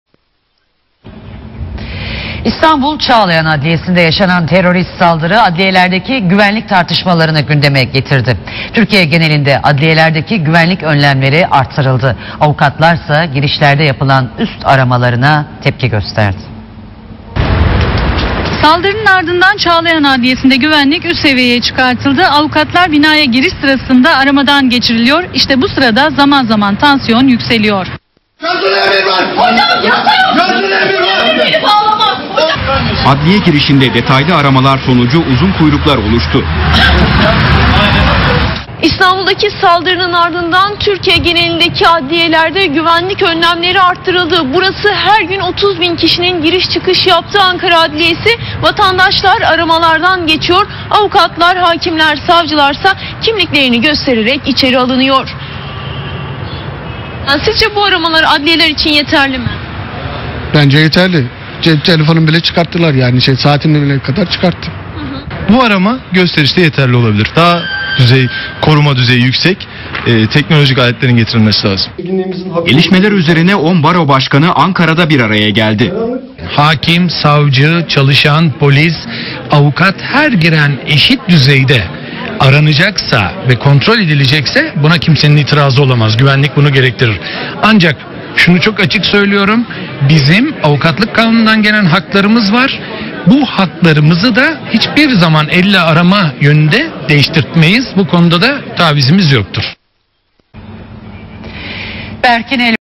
Ankara Barosu Başkanı Av Hakan Canduran, TRT Haber'in, Çağlayan Adliyesi'ndeki terör eyleminin ard
Ankara Barosu Başkanı Av. Hakan Canduran, TRT Haber'in, Çağlayan Adliyesi'ndeki terör eyleminin  ardından avukatların üstünün aramasına ilişkin sorularını yanıtladı